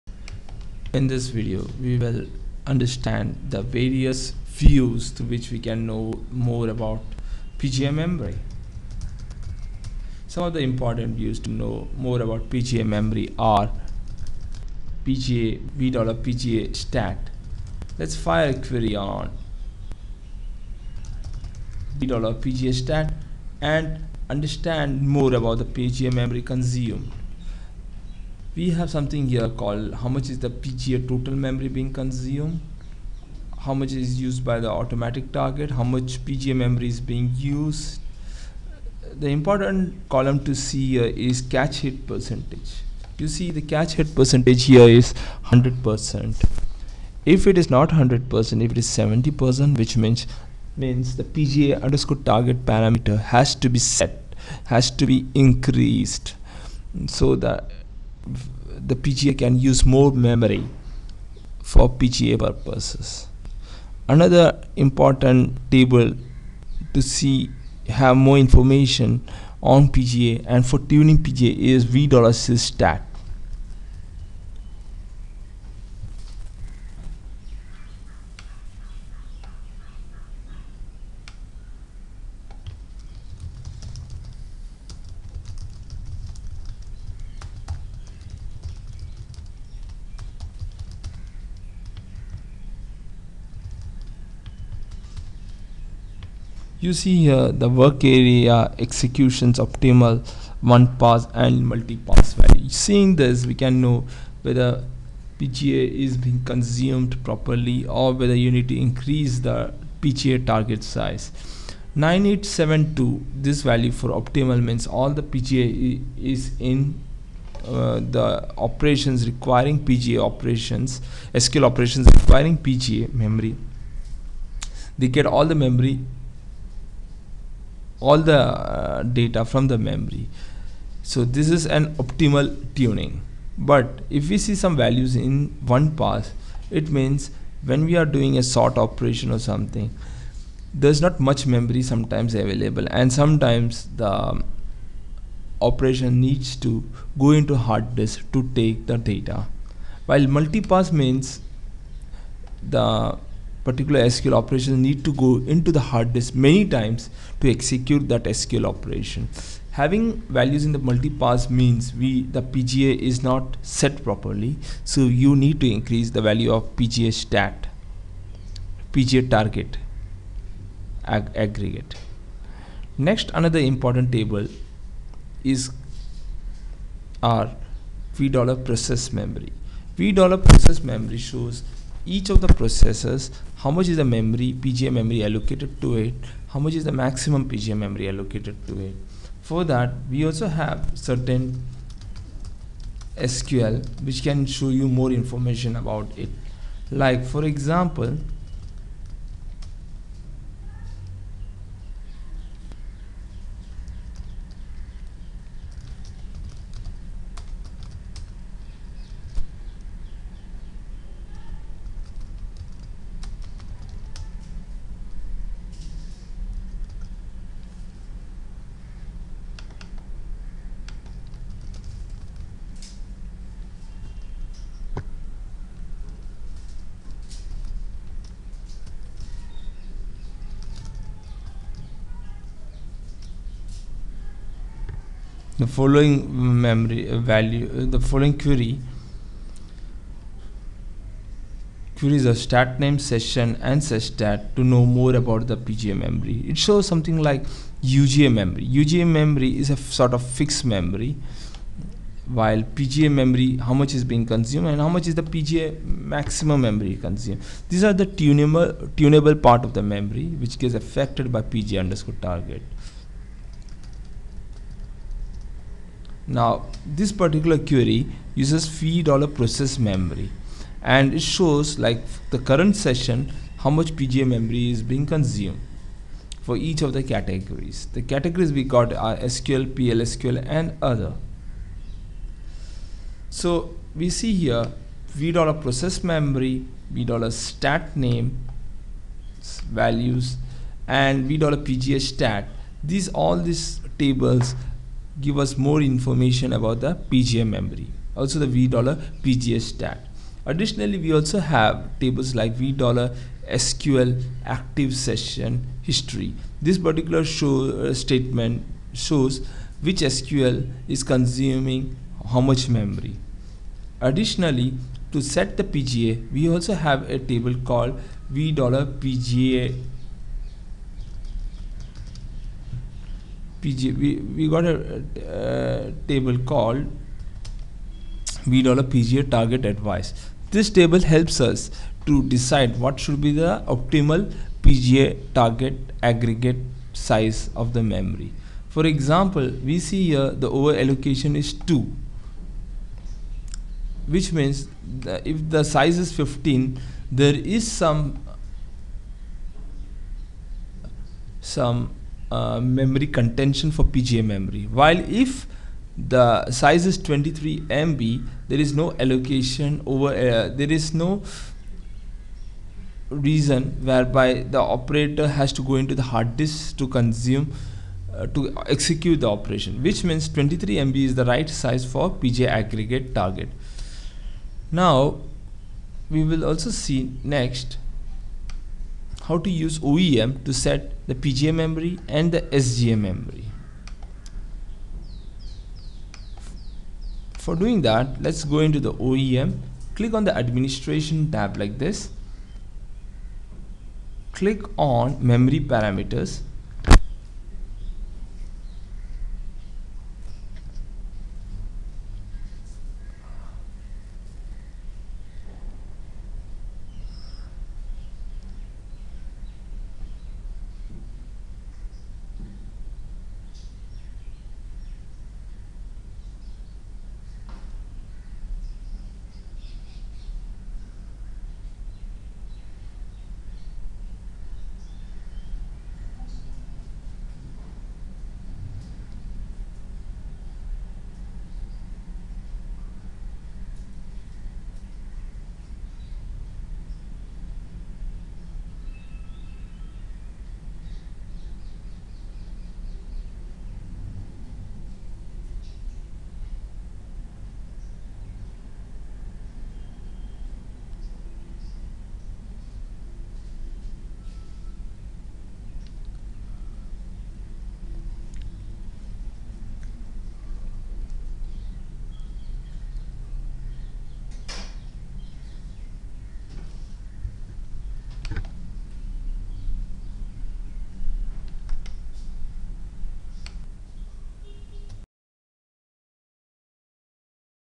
oracle pga tuning ,Oracle program global area tuning
oracle pga needs to be tuned for oracle tuning
see some important views related to oracle tuning